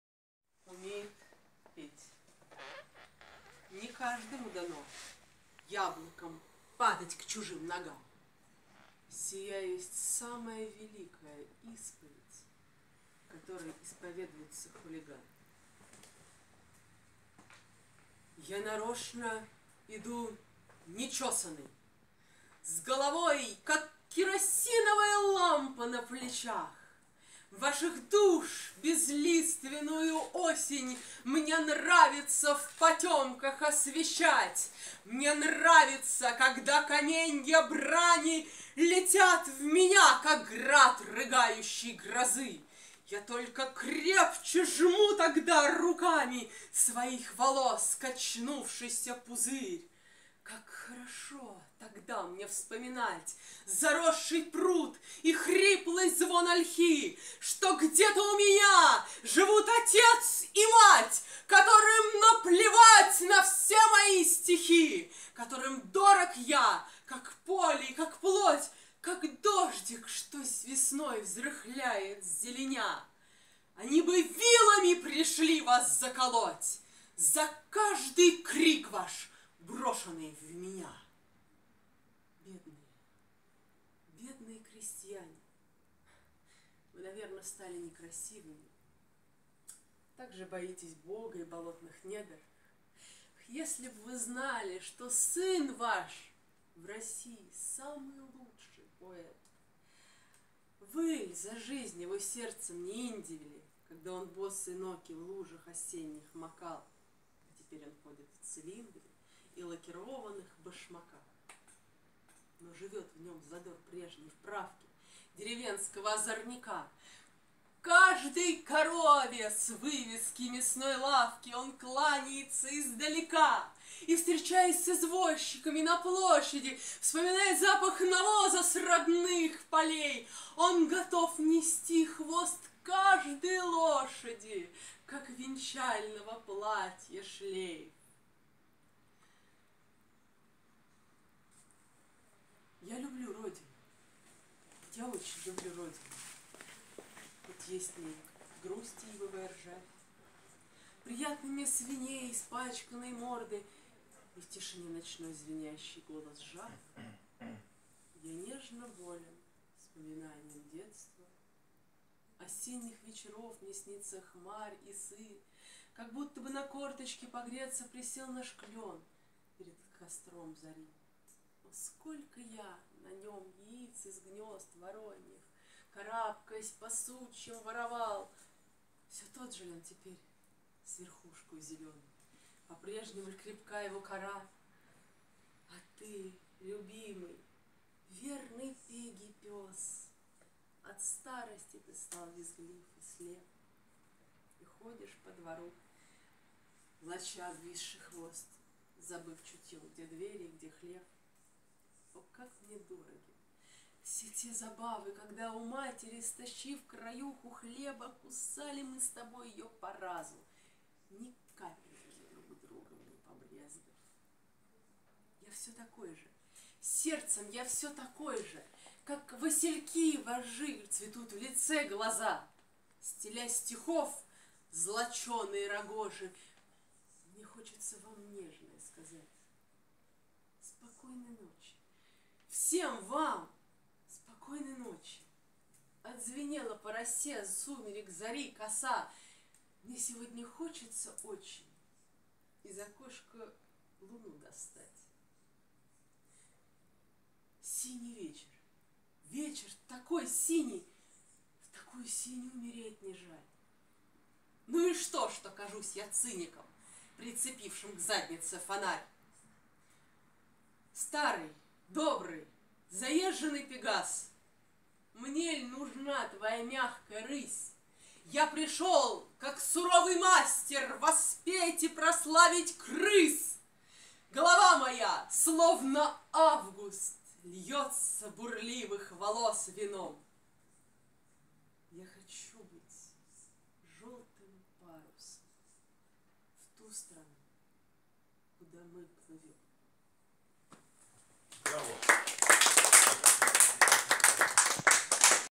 Ирина Марго - Исповедь Хулигана, стихи Сергей Есенин Библиотека Вересаева
Ирина Марго - Исповедь Хулигана, стихи
 Сергей Есенин
Библиотека Вересаева
Арт-Салон Мастер
"Заметался пожар голубой" концерт в честь дня рождения Сергея Есенина
5 октября 2017